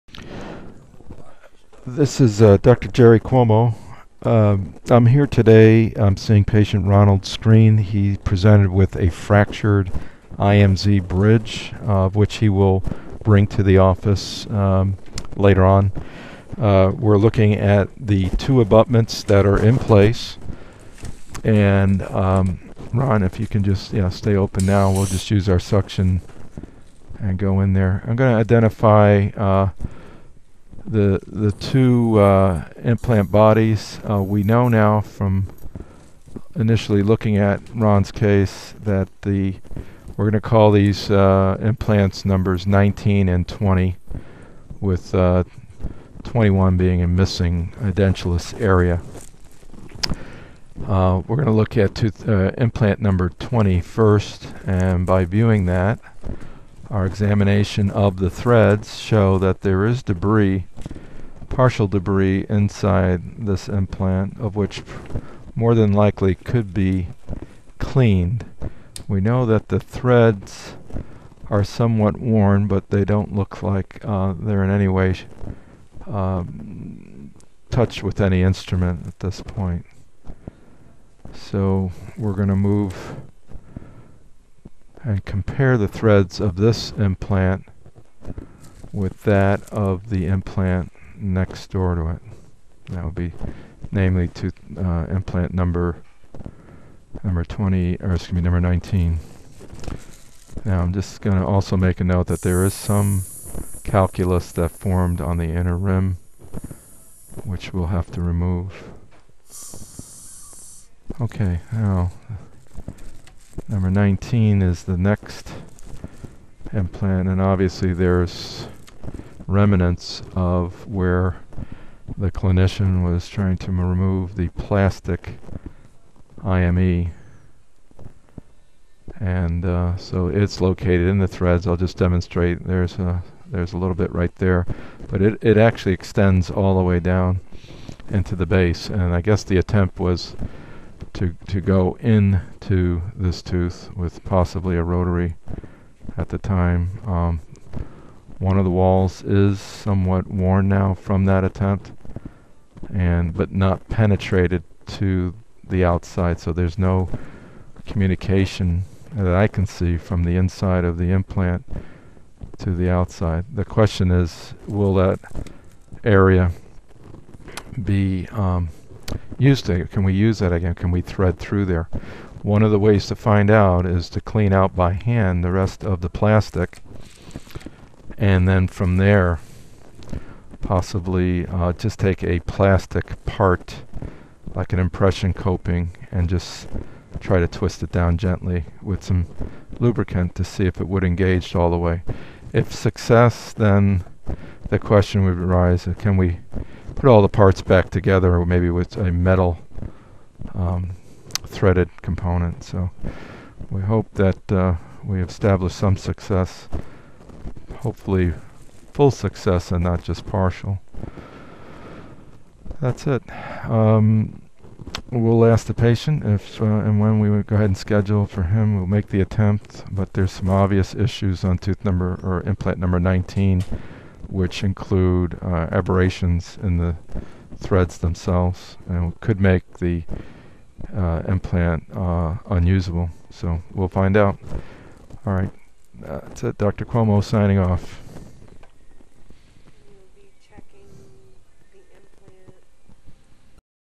DR.GERARD CUOMO -Dental Implant IMZ case remove a fractured IME using a microscope Boca Raton, FL
PART 1 This case was referred to my office to salvage a historic IMZ implant body.  Patient presented with a three tooth fixed implant cantilever bridge #19 -- #21 completely out of the mouth.  IMZ implants occupied the #19 and #20 positions.  The patient's dentist unsuccessfully attempted to remove the IME from implant body in toth position #19.  The dentist decided to refer the patient my office for an implant rescue procedure performed by using a dental microscope.  This segment includes and initial diagnosis and plan.